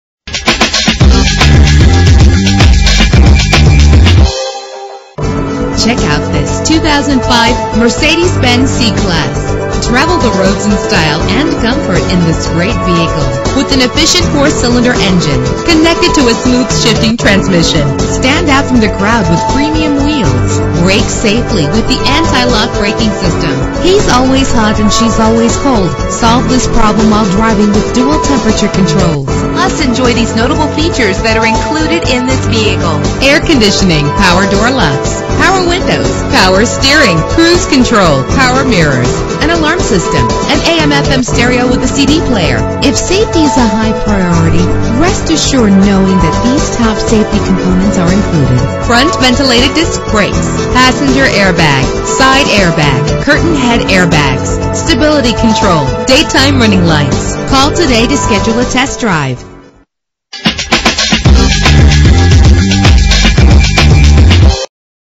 2005 Mercedes-Benz C-Class - Huntersville NC
Year: 2005
Make: Mercedes-Benz
Model: C-Class
Trim: C230 Kompressor Sport
Engine: 1.8 liter inline 4 cylinder 16 valve
Transmission:
Color: Black
Mileage: 82919
Address:
13825 Statesville Rd